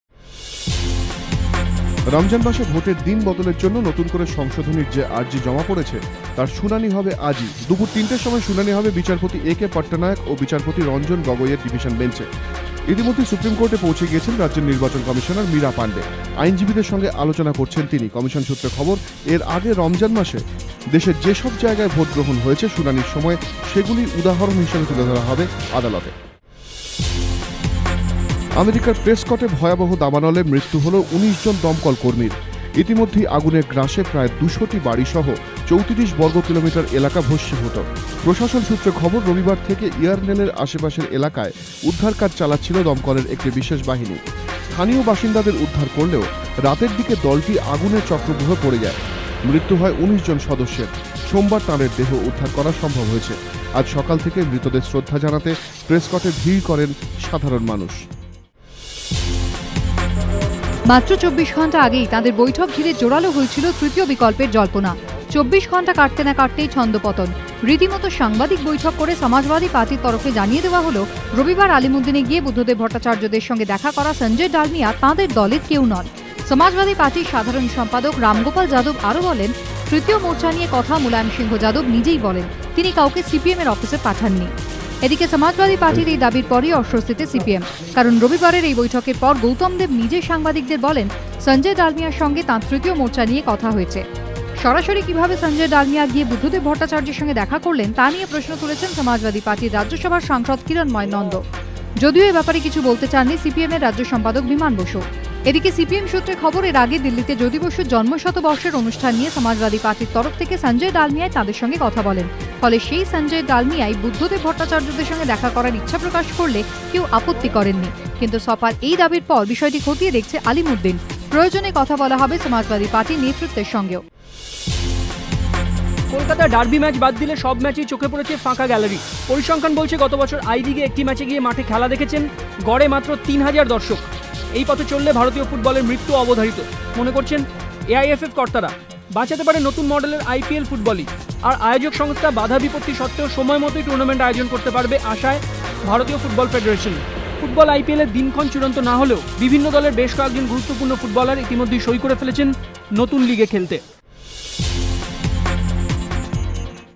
3 মিনিটে এই মুহুর্তের বড় খবর
west bengal government's plea for review seeking change in panchayat poll dates will be heard today. The election commision is likely to present the examples where polls were held during ramzan.